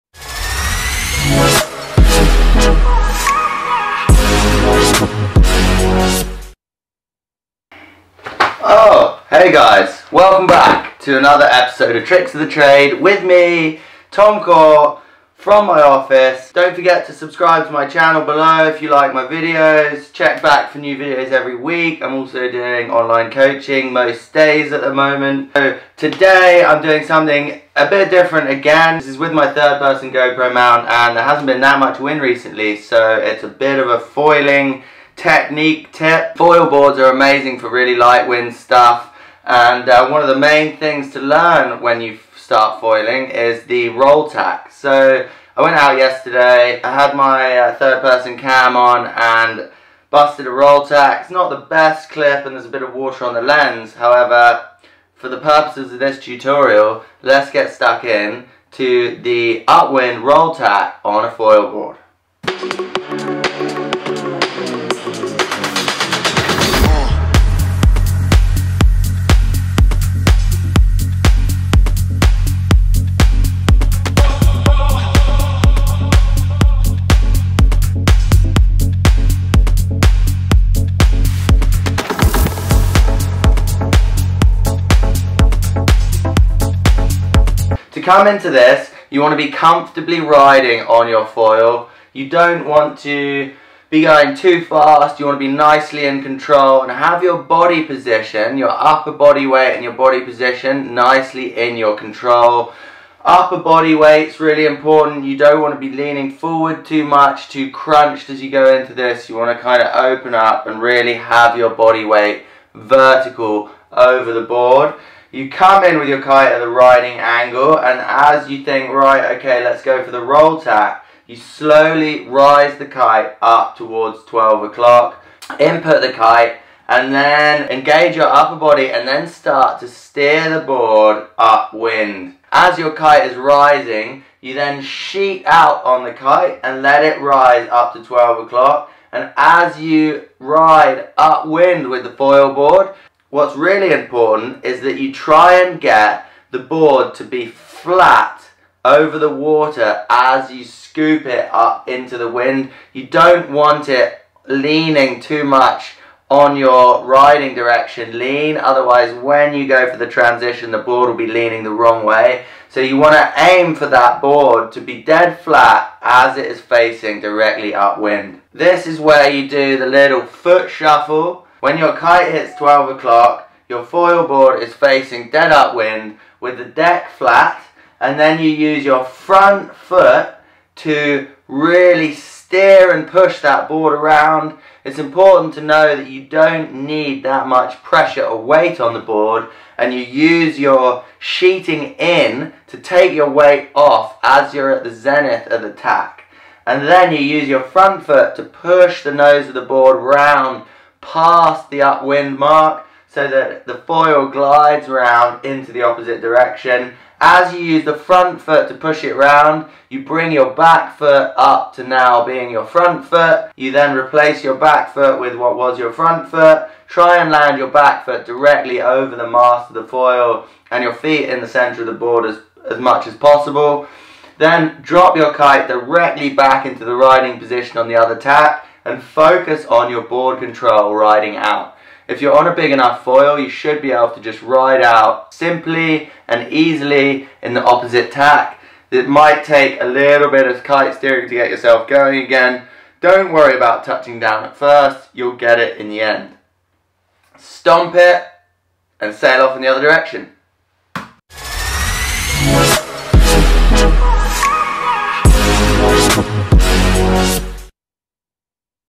How to "Tack" on a Foil Board - Kiteboarding Tricks of the Trade with Tom Court
In this episode of tricksofthetrade I look at a 3rd person clip of a roll tack from my last session on my @DUOTONEKiteboarding 1250 spirit range foil board. It isn't a great clip as there is some water on the lens, however I feel that with this camera angle you can benefit from the kite control and also the timing that you can clearly see...

This is one of the first things to learn of a hydrofoil as it is the most efficient way to turn around without loosing down wind and will always come in handy on your journey to learn how to foil. Kite foiling is best in lighter winds and increases your ridable wind range by almost double... so it is well worth learning!!

For more information on my slice of life kite coaching trips with @KiteWorldWide check out this